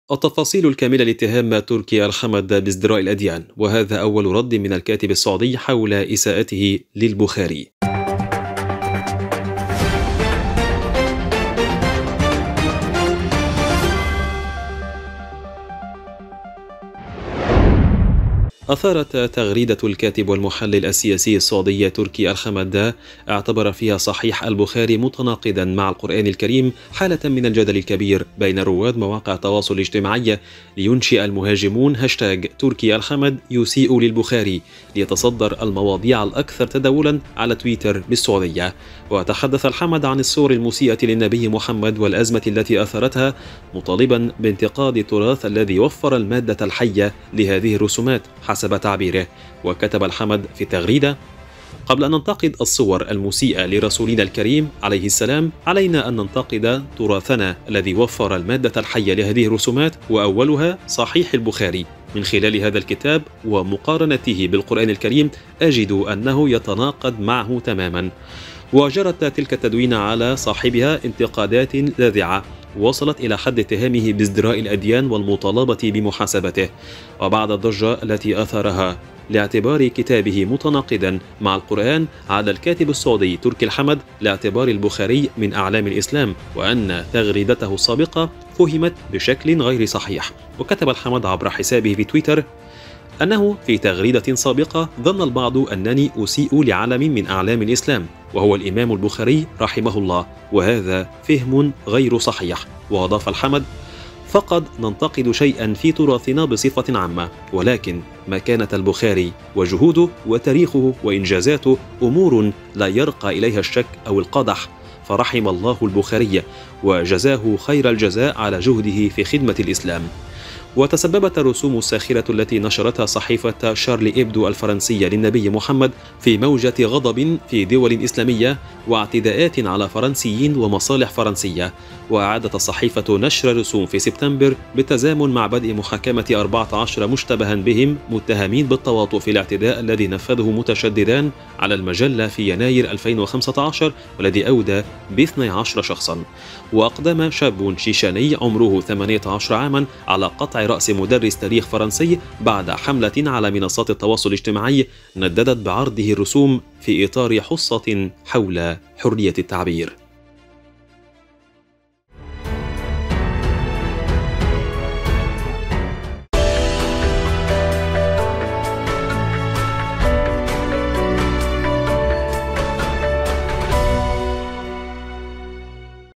التفاصيل الكاملة لاتهام تركي الحمد بازدراء الأديان.. وهذا أول رد حول إساءته لـ "البخاري"!
أثارت تغريدة الكاتب والمحلل السياسي السعودي تركي الحمد، اعتبر فيها صحيح البخاري متناقضا مع القرآن الكريم، حالة من الجدل الكبير بين رواد مواقع التواصل الاجتماعي، لينشئ المهاجمون هاشتاج "تركي الحمد يسيء للبخاري" ليتصدر المواضيع الأكثر تداولا على تويتر بالسعودية.
وتحدث الحمد عن الصور المسيئة للنبي محمد والأزمة التي أثارتها، مطالبا بانتقاد "التراث الذي وفر المادة الحية لهذه الرسومات"، حسب تعبيره.
وكتب الحمد في التغريدة: "قبل أن ننتقد الصور المسيئة لرسولنا الكريم، عليه السلام، علينا أن ننتقد تراثنا الذي وفر المادة الحية لهذه الرسومات، وأولها صحيح البخاري.. من خلال هذا الكتاب، ومقارنته بالقرآن الكريم، أجد أنه يتناقض معه تماما."
وجرت تلك التدوينة على صاحبها انتقادات لاذعة، وصلت إلى حد اتهامه بازدراء الأديان والمطالبة بمحاسبته.
وبعد الضجة التي أثارها لاعتبار كتابه متناقضا مع القرآن، عاد الكاتب السعودي تركي الحمد لاعتبار البخاري من أعلام الإسلام وأن تغريدته السابقة "فهمت بشكل غير صحيح".
وكتب الحمد، عبر حسابه في تويتر، أنه "في تغريدة سابقة، ظن البعض أنني أسيء لعلم من أعلام الإسلام، وهو الإمام البخاري رحمه الله، وهذا فهم غير صحيح".
وأضاف الحمد: "فقد ننتقد شيئا في تراثنا بصفة عامة، ولكن مكانة البخاري وجهوده، وتاريخه، وإنجازاته، أمور لا يرقى إليها الشك أو القدح، فرحم الله البخاري، وجزاه خير الجزاء على جهده في خدمة الإسلام".
وتسببت الرسوم الساخرة التي نشرتها صحيفة شارلي إيبدو الفرنسية للنبي محمد في موجة غضب في دول إسلامية، واعتداءات على فرنسيين ومصالح فرنسية.
وأعادت الصحيفة نشر الرسوم في سبتمبر بالتزامن مع بدء محاكمة 14 مشتبها بهم متهمين بالتواطؤ في الاعتداء الذي نفذه متشددان على المجلة في يناير 2015 والذي أودى ب12 شخصا.
وأقدم شاب شيشاني عمره 18 عاما، على قطع رأس مدرس تاريخ فرنسي، بعد حملة على منصات التواصل الاجتماعي نددت بعرضه الرسوم في إطار حصة حول حرية التعبير.